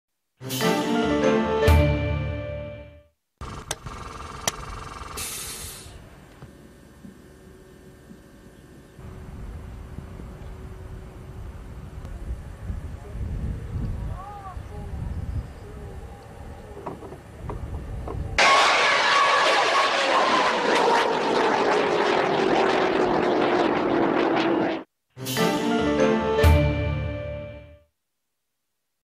Rocket Lab : Ātea-1 Suborbital Rocket Launch (short version)
Atea-1 rocket launch on November 30, 2009 by Rocket Lab from Great Mercury Island, New Zealand. Short version.